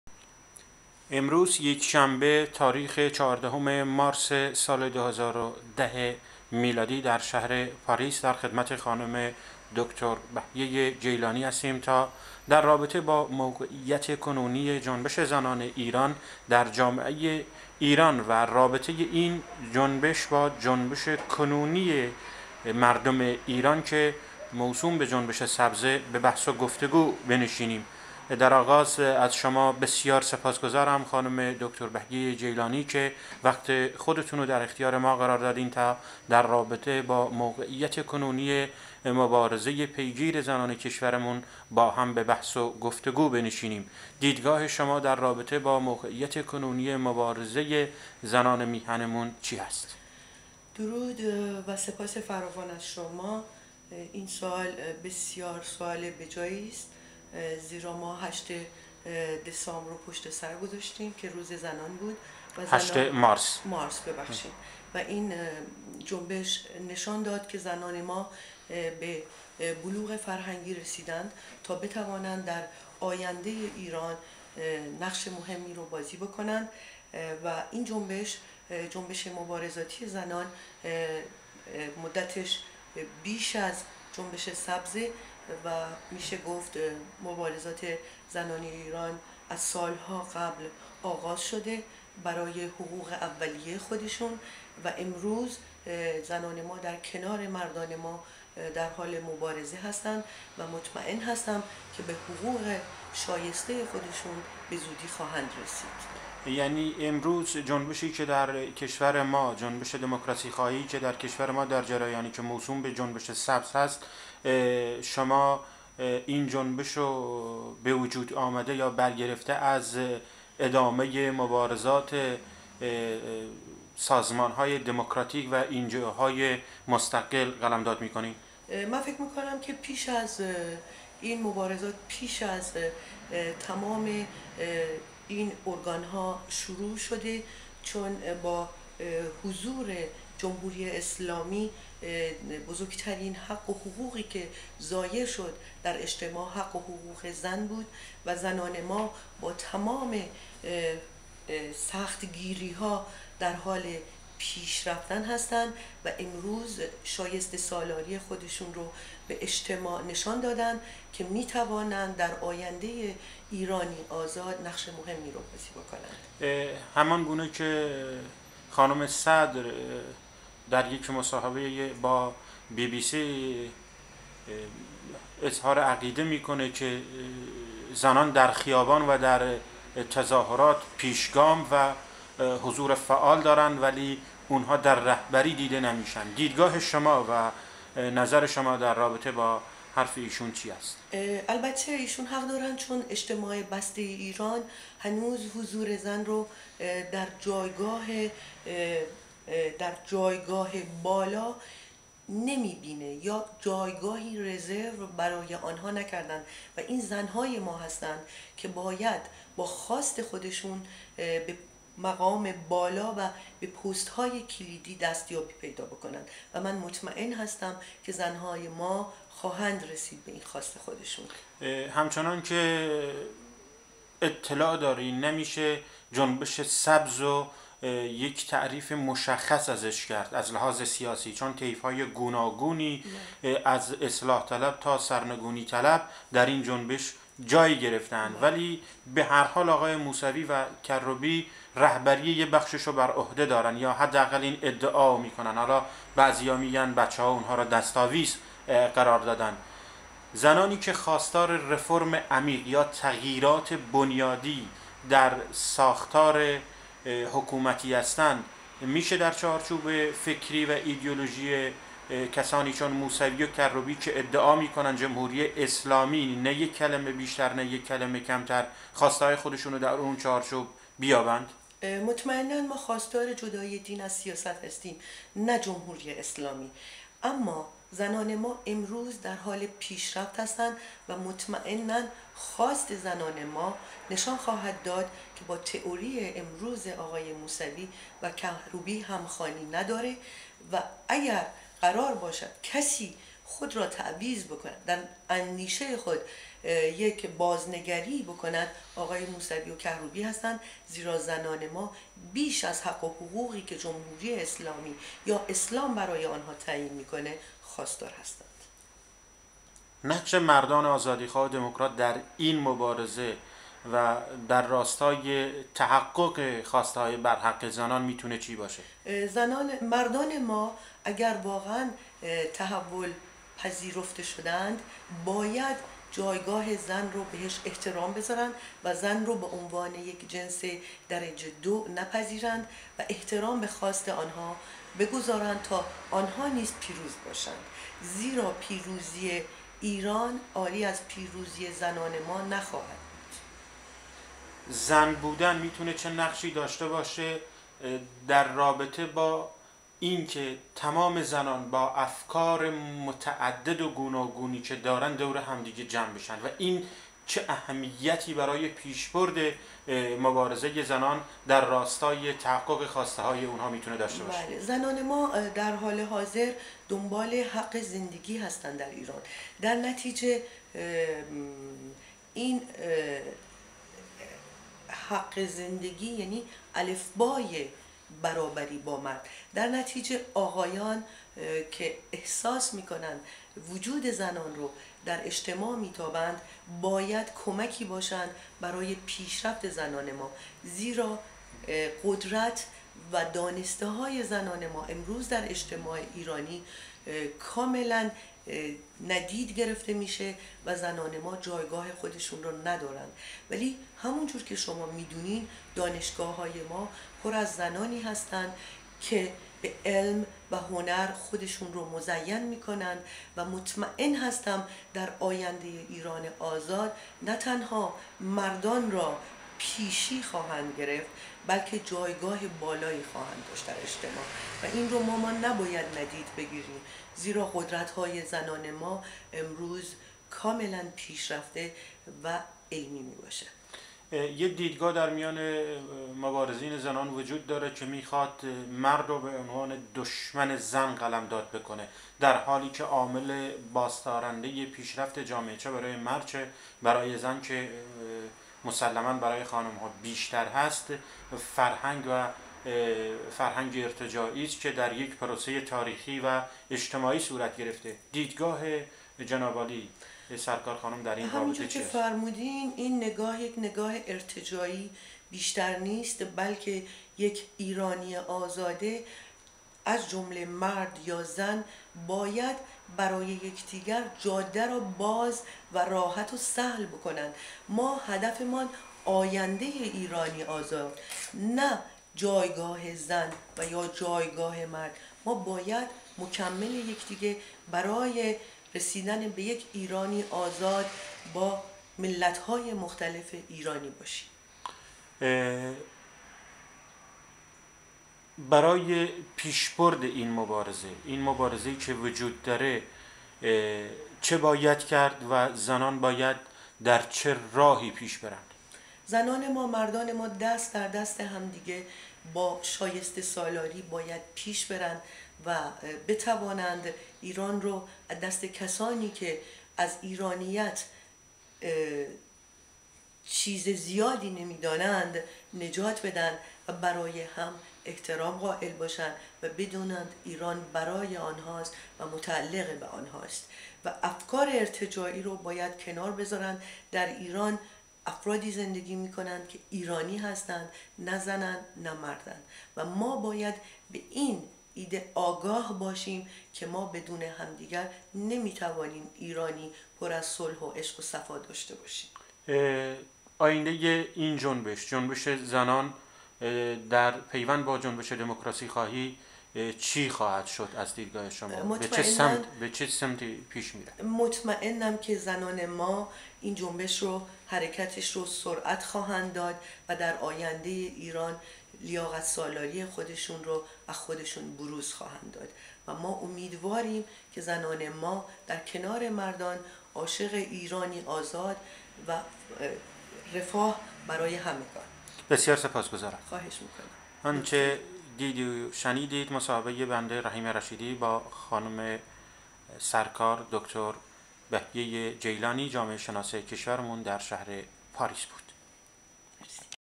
گفتگوی رحیم رشیدی با دکتر بهیه‌ جیلانی در مورد زنان
گفتگوی رحیم رشیدی با دکتر بهیه‌ جیلانی در مورد موقعیت کنونی جنبش زنان در ایران